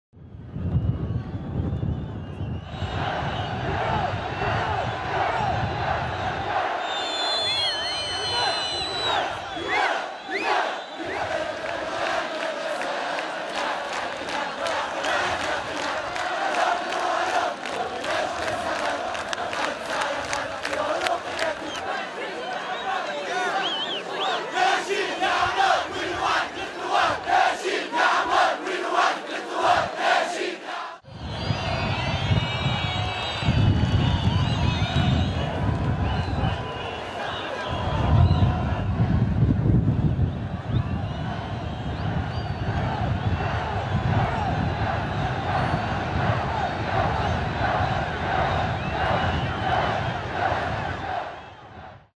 Thousands protest in Tunis
Thousands of protesters demonstrate in Tunis calling for the removal of Prime Minister Mohamed Ghannouchi.